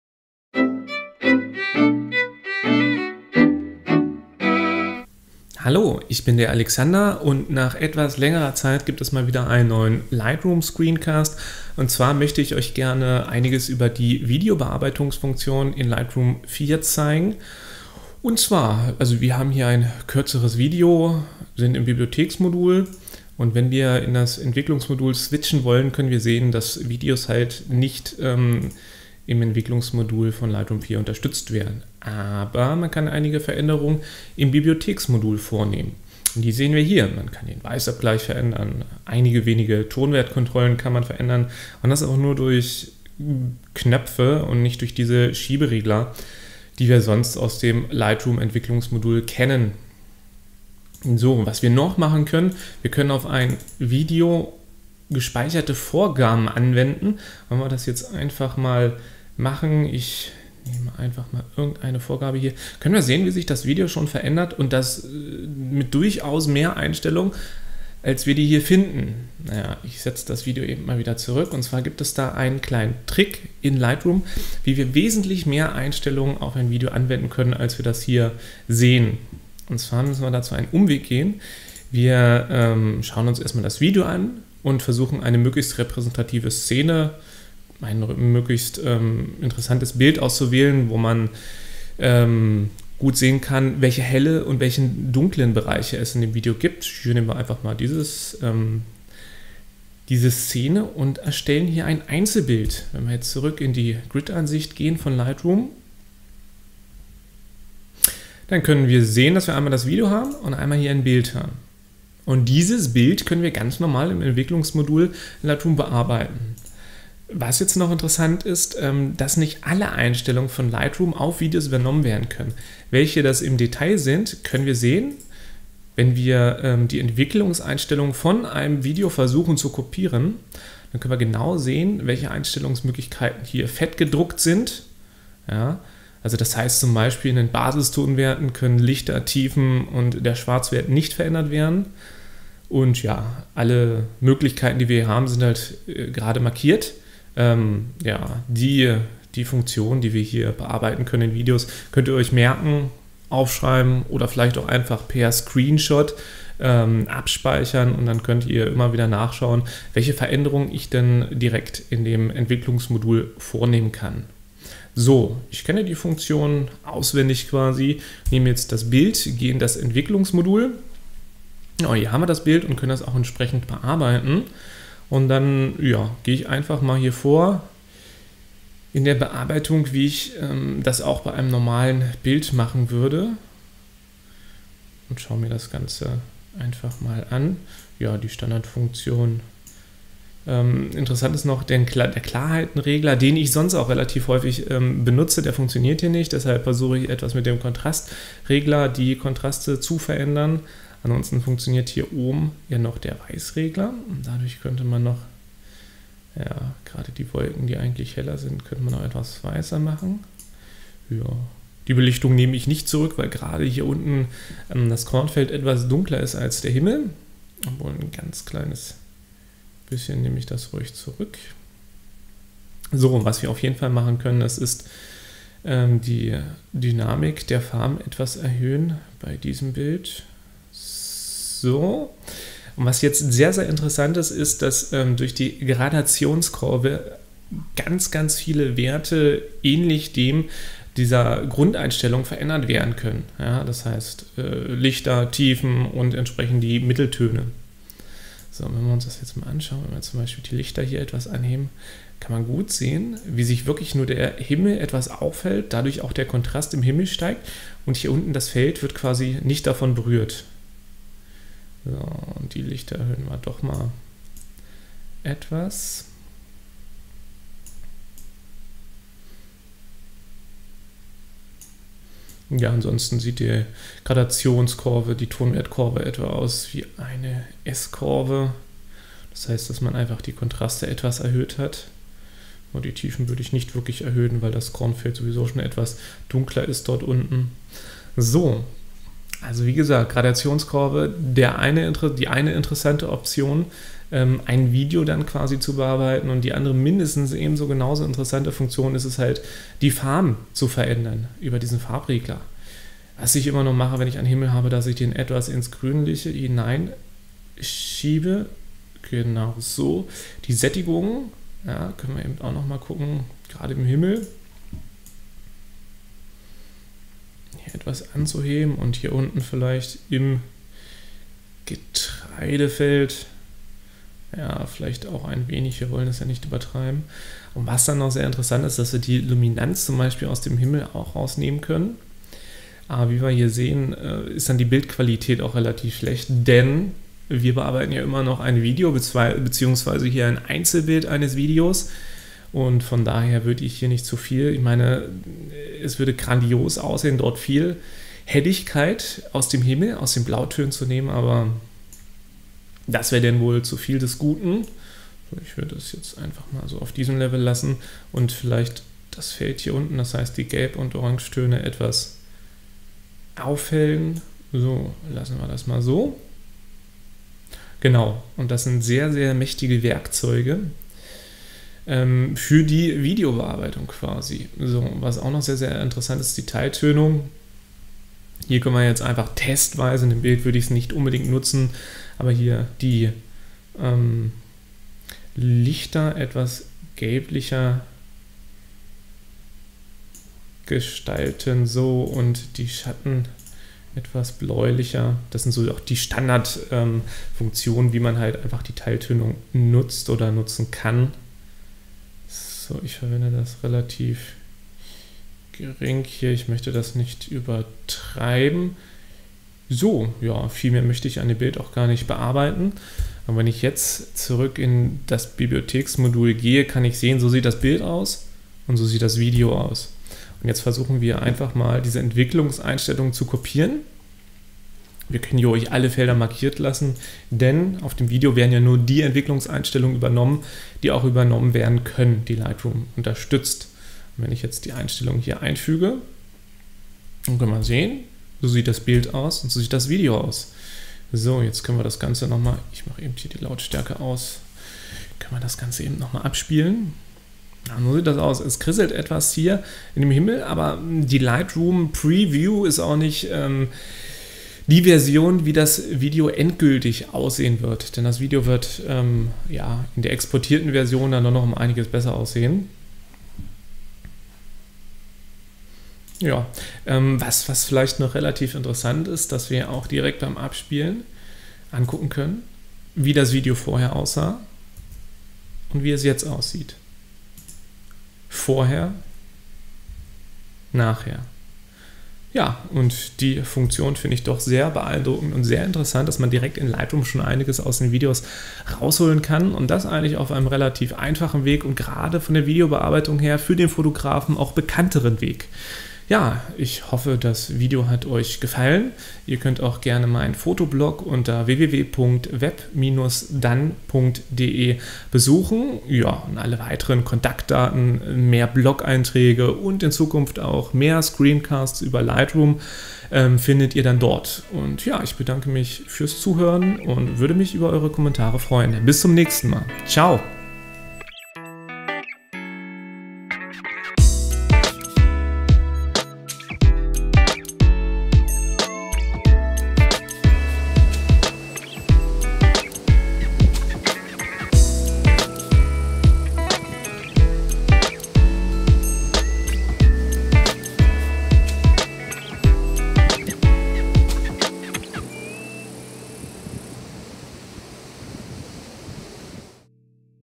Lightroom 4 Tutorial - Erweiterte Videobearbeitung
Lightroom 4 bietet nativ nur wenige Möglichkeiten Videos zu bearbeiten. Ein paar Knöpfe im Bibliotheks-Modul und das war es. Aber durch einen kleinen Umweg lassen sich Videos ähnlich wie Bilder bearbeiten. Hierzu ist es notwendig ein Einzelbild aus dem Video zu erstellen und dieses im Enwicklungs-Modul zu bearbeiten. Die angewendeten Einstellungen können dann auf das Video übernommen werden. Jedoch werden nicht alle Einstellungen im Video sichtbar. Das Folgende Video zeigt im detail, wie Videos in Lightroom bearbeitet werden können.